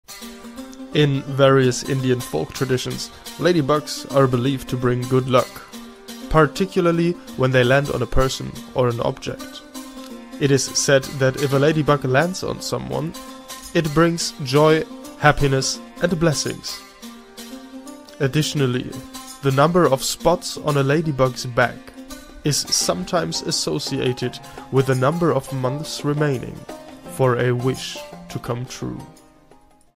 The Ladybug in India
Embark on a captivating exploration of Indian folklore as we uncover the fascinating relationship between the ladybug and the rich tapestry of beliefs in Indian culture. Join us on a spiritual journey that reveals the significance of these enchanting insects in the traditions and mythologies of India.

In this enlightening video, we delve into the depths of Indian folklore and discover the spiritual symbolism attributed to ladybugs. Unveil the tales that illustrate the ladybug as a symbol of good fortune, prosperity, and divine blessings in Indian beliefs.

Through captivating storytelling, we'll explore the ways in which ladybugs are considered auspicious creatures, believed to bring luck and happiness to those they encounter. We'll unravel the cultural significance of the ladybug's presence in homes, gardens, and sacred spaces, where they are welcomed as signs of divine favor.

Witness the beauty of Indian traditions and their profound connection to the natural world as we explore the spiritual teachings that highlight the ladybug's role as a messenger of positivity and harmony. Discover how these vibrant insects have woven their way into the fabric of Indian culture, inspiring reverence and gratitude for the interconnectedness of all life.

Join us on this immersive journey into Indian folklore, where the ladybug serves as a bridge between the earthly and the spiritual realms, reminding us of the sacredness of nature and the blessings that abound. Let the wisdom of Indian traditions inspire you and deepen your appreciation for the spiritual significance of these beloved creatures.